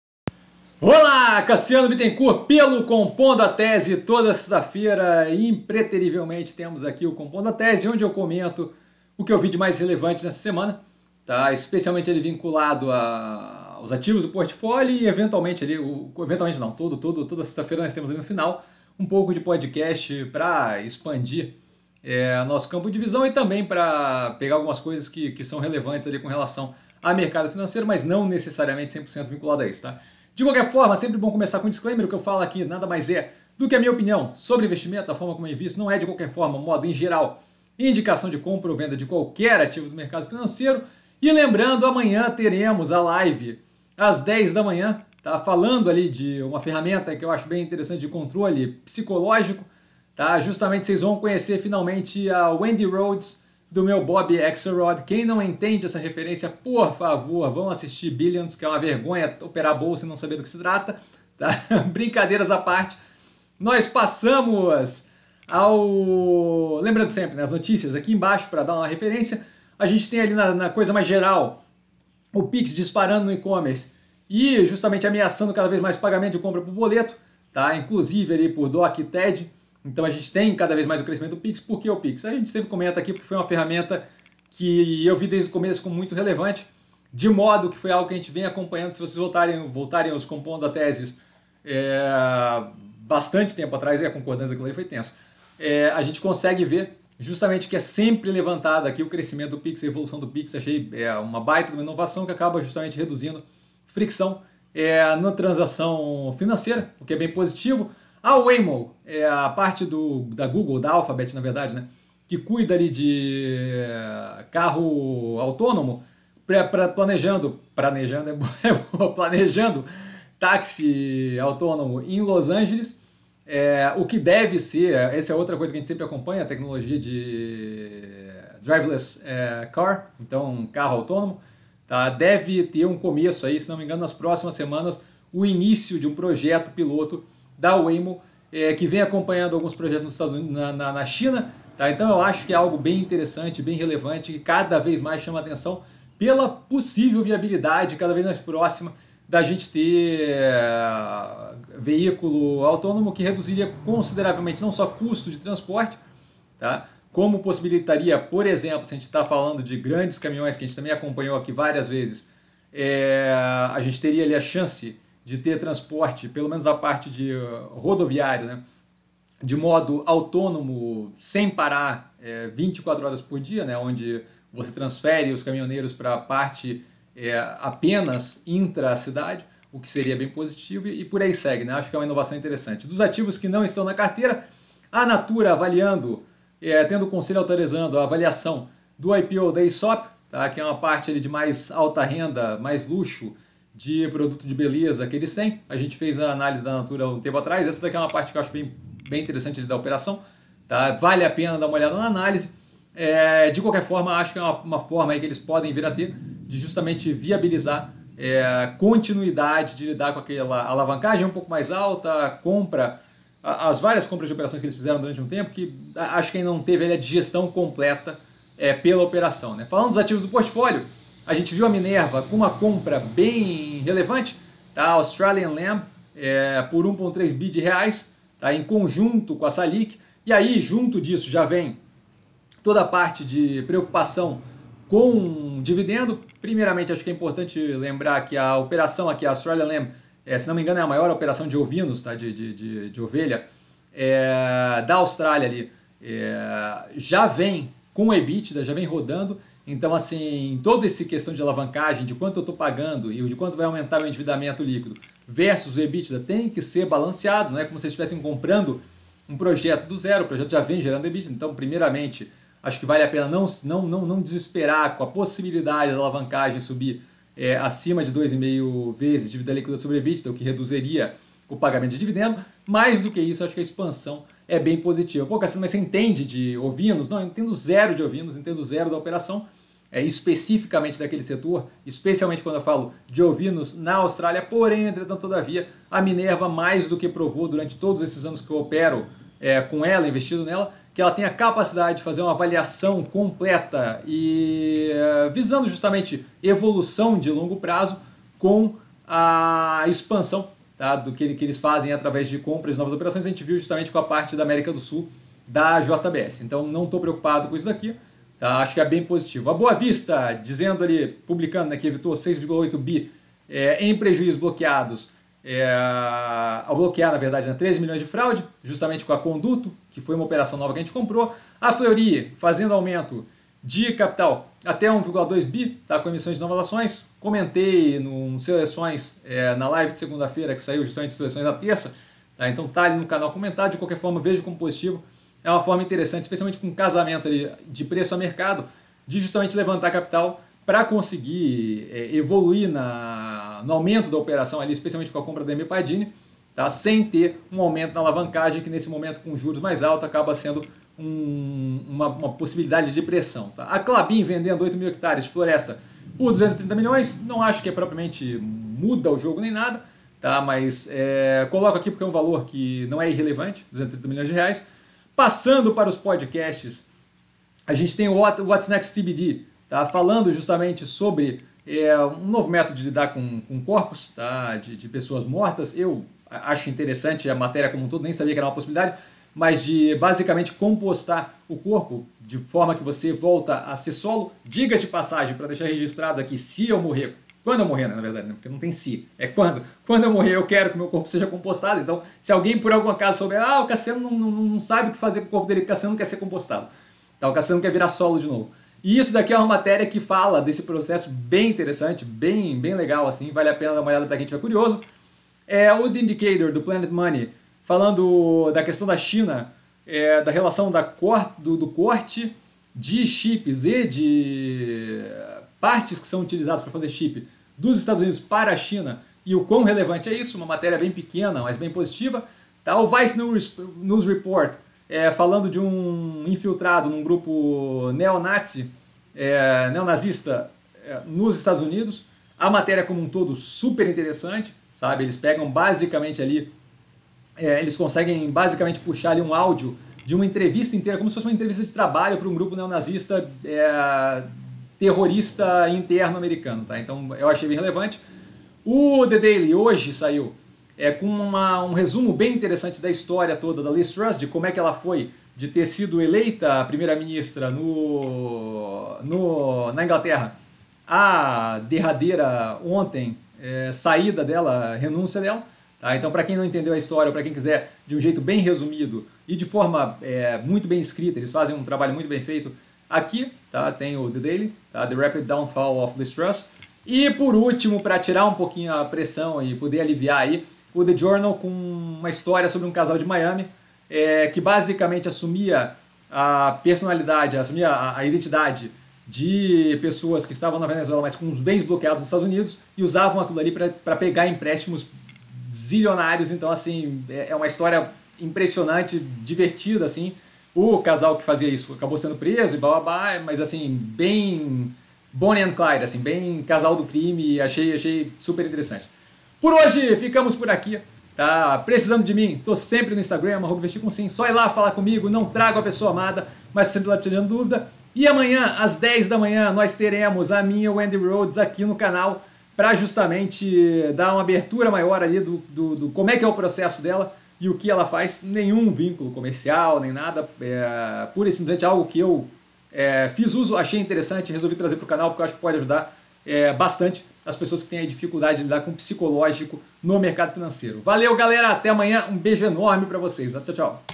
21.10.2022 - Compondo a Tese - 14/10/22 a 21/10/22
Pix dispara no e-commerce e deve decretar fim de pagamentos de compras por boleto

Waymo plans to launch driverless taxi service in Los Angeles

Natura (NTCO3): Conselho autoriza estudo para avaliar realização de IPO ou cisão da Aesop

Minerva (BEEF3) compra frigorífico Australian Lamb por R$ 1,3 bilhões, confirma companhia

Boa Vista diz que evitou R$ 6,8 bi em prejuízo ao bloquear 13 milhões de fraudes

Fleury fará aumento de capital de até R$ 1,2 bi com emissão de novas ações

Klabin (KLBN11) vende 8 mil hectares de floresta por R$ 230 milhões

What Next TBD: Are You Ready to Be Composted When You Die?

Why the US cut China off from advanced chips

How a Miami Couple Used Empty Mansions to Pocket Millions